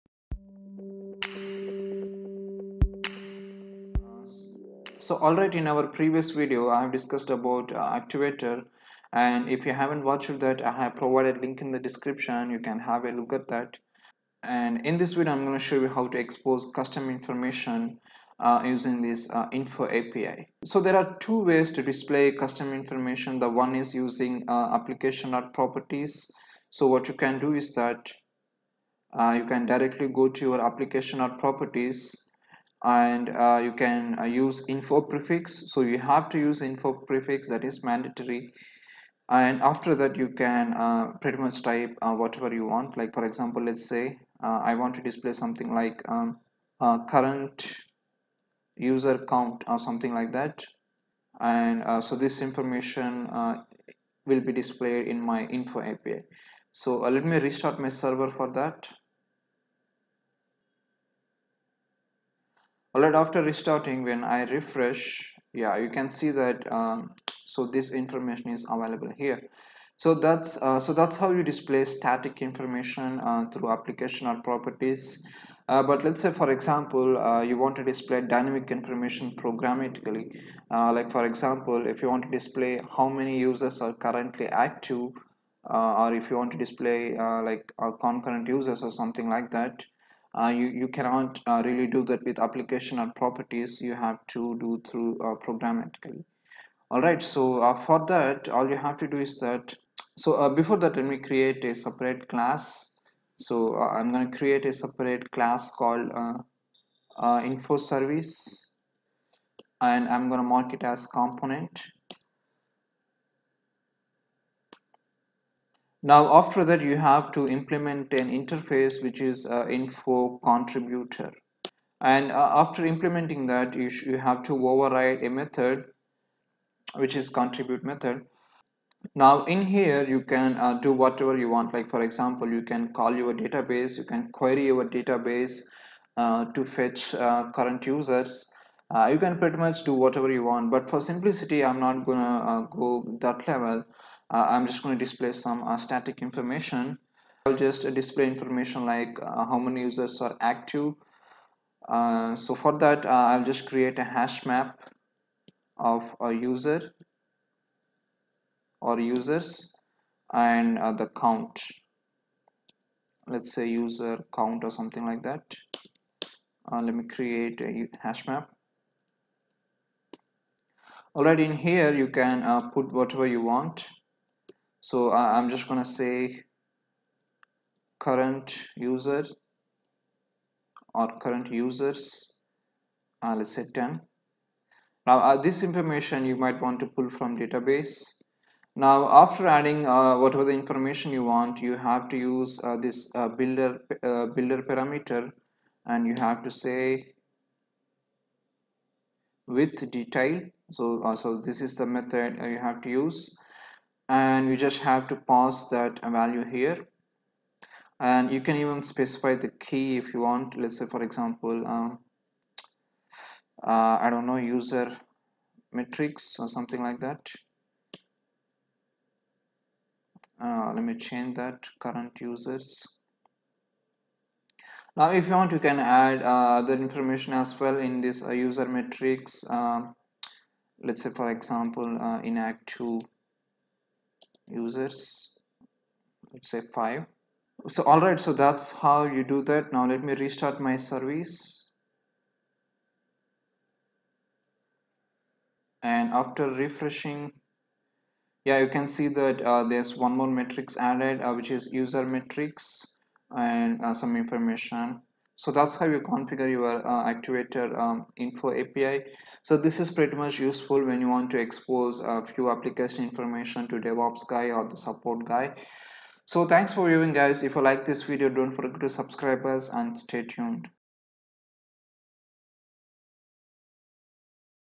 Spring Boot Actuator Custom Metrics With INFO API
This Video Covers How to Display Custom Information in Actuator using INFO API End Point.

 There are two ways to display custom metrics

1. Using application.properties to display static information
2.  By extending Interface to display dynamic information